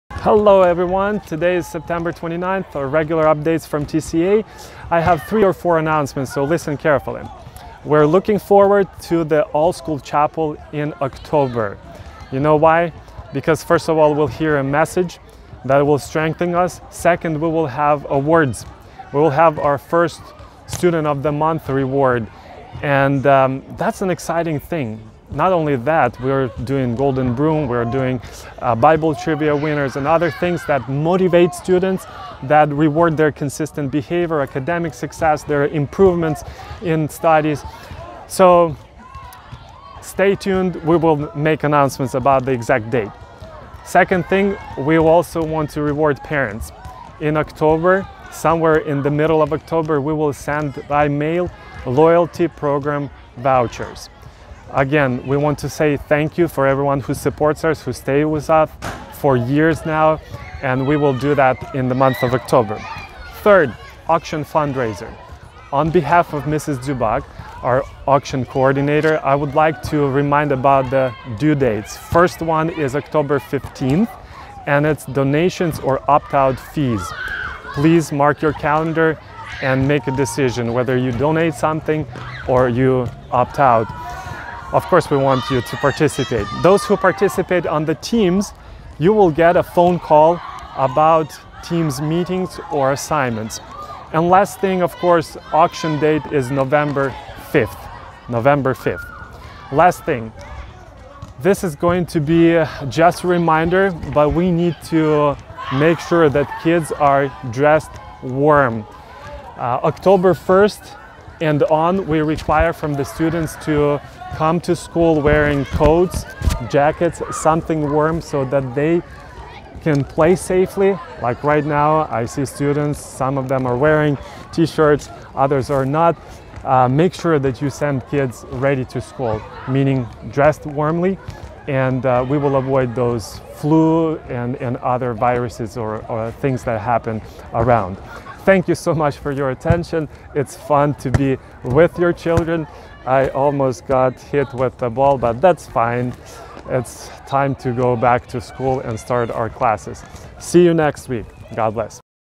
2 TCA News Sept 29, 2021
Announcements:
1. Rewarding Students
2. Loyalty Program Vouchers - next month
3. School Auction
       --Auction: Nov 5
4. Stay Healthy: Dress Warm